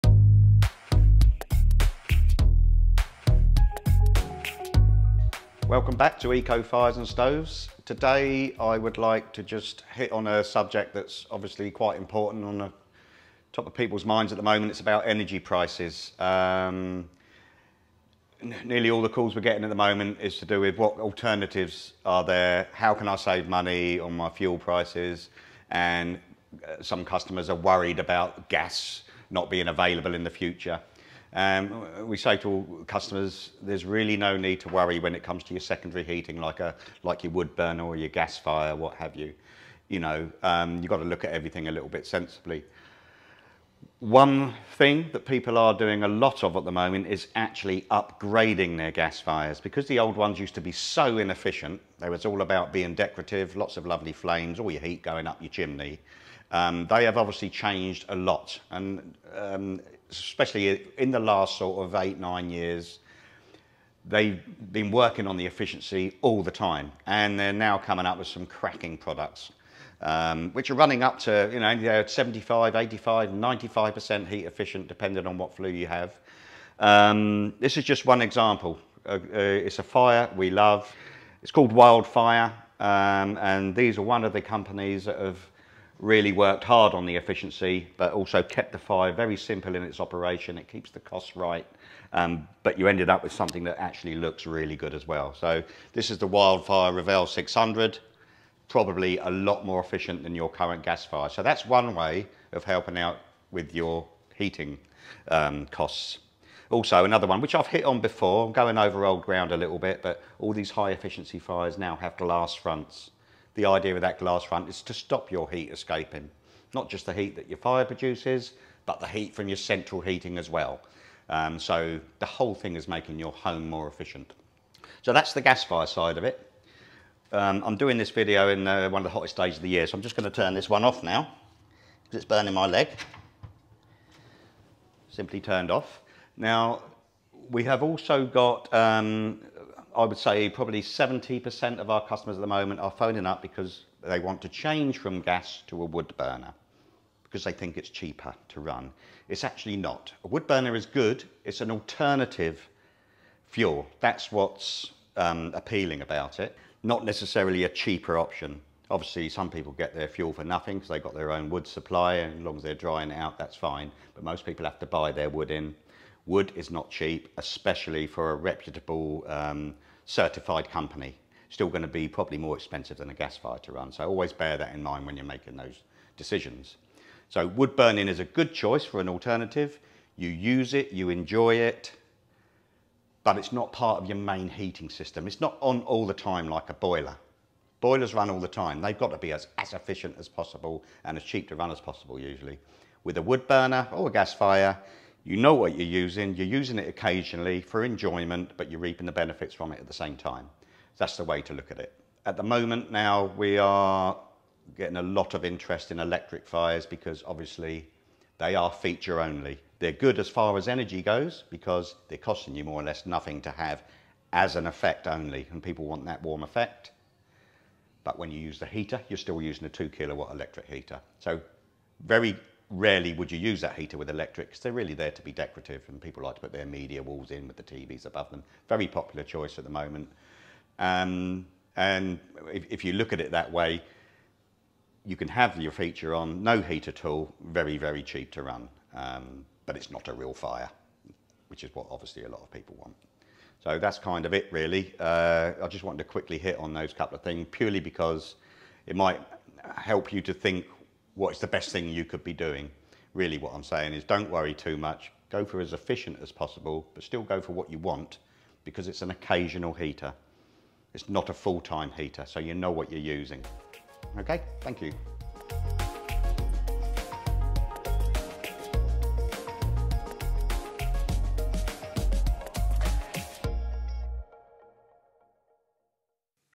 Energy Crisis Advice For Wood Burners Gas Fires and Electric Fire Owners
In this video Pete Hillier a Director of EcoFires and Stoves in Fleet Hampshire UK gives advice on choosing the right fire during the energy crises.

There are lots of things to consider, but most importantly as pete says  "There really is no need to worry when it comes to your secondary fires, like the wood burners, gasfires or ecectric stoves, because they are 'Secondary Heating appliances".

One thing a lot of people are doing at the moment are upgrading their gas fires, because many of the older gas fires are not very efficient. The newer ones have glass fronts on them to help contain the heat they create, meaning that you can now purchase a gas fire that is up to 95% heat efficient.

Pete Hillier is an expert on Wood Burning Stoves and Gas and Electric Stoves